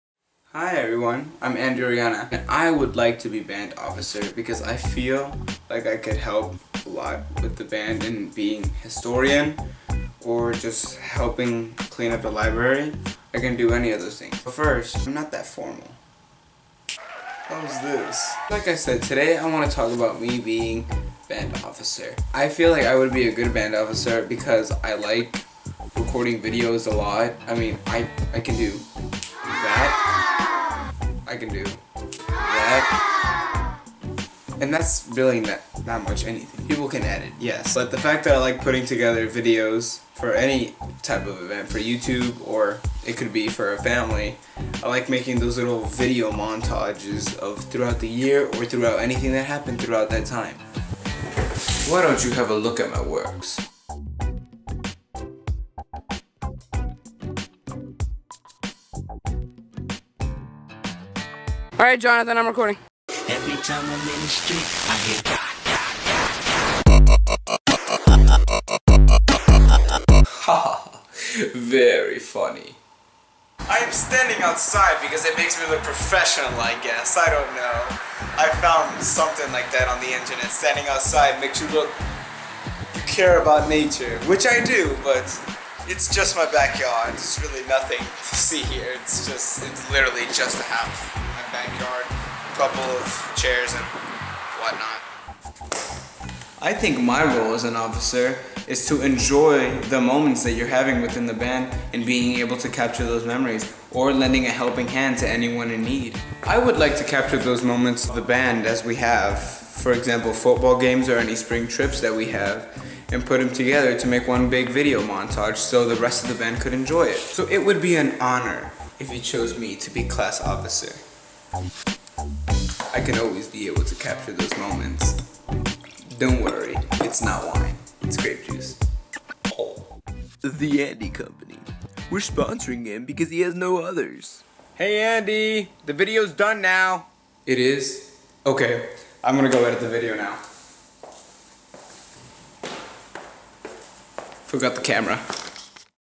Andy Orellana's Band Officer Tryout Video
This video is for my Tryout placement in the Obra D. Tompkins High School Band.